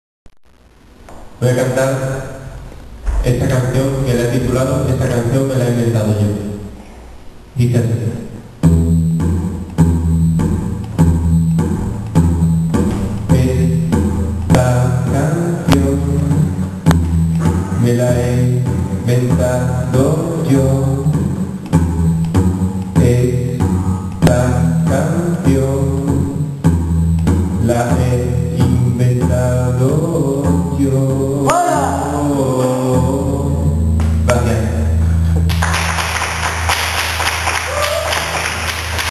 el cantautor johnny moon
esta cancion me la he inventado yo